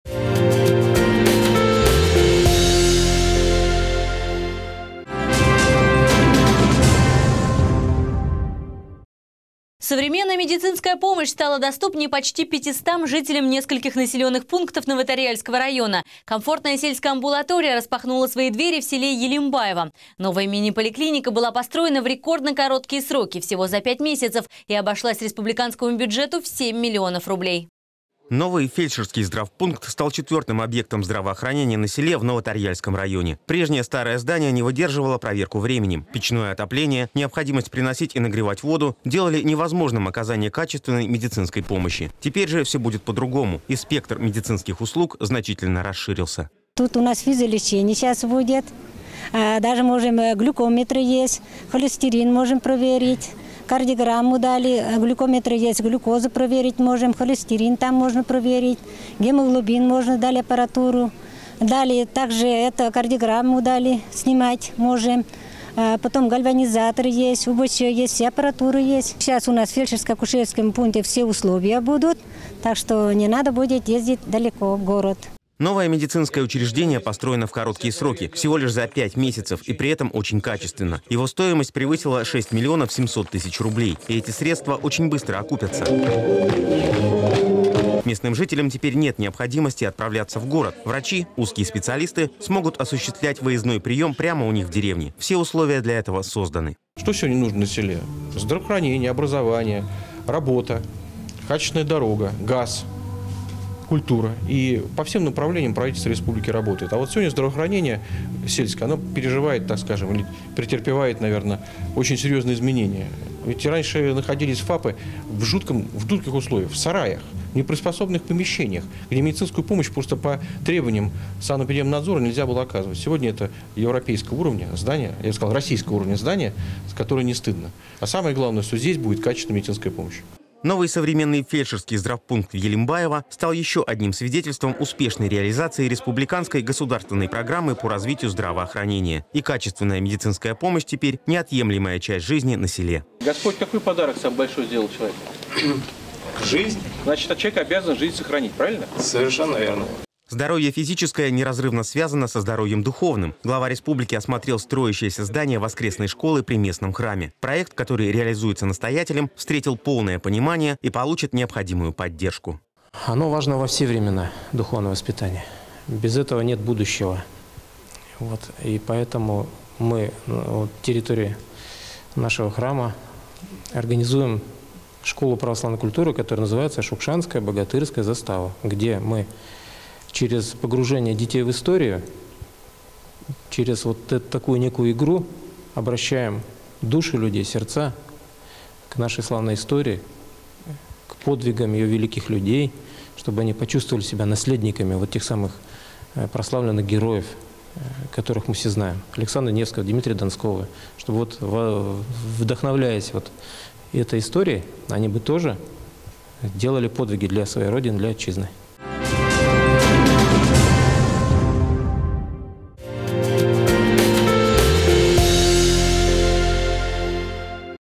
Открытие сельской амбулатории в Новоторъяльском районе Марий Эл
Современная медицинская помощь стала доступнее почти 500 жителям нескольких населенных пунктов Новоторъяльского района!

Комфортная сельская амбулатория распахнула свои двери в селе  Елембаево. Новая мини-поликлиника была построена в рекордно-короткие сроки - всего за пять месяцев, и обошлась республиканскому бюджету в семь миллионов рублей.